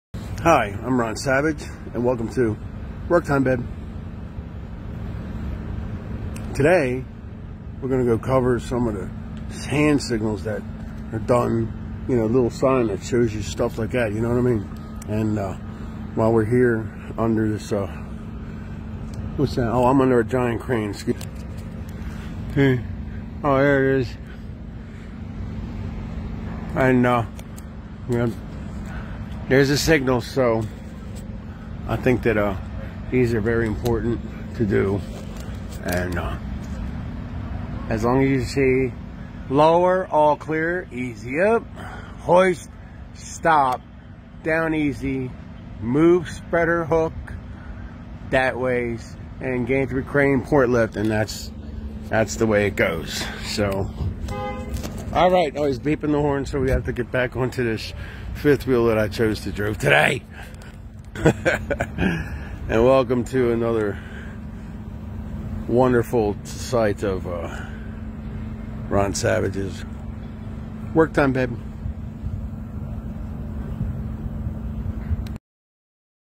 RON SAVAGE: Ron Savage's "Work Time Babe ". Hand signal this!
Here under a giant crane on the docs eye show you secret hand signals for you to live by.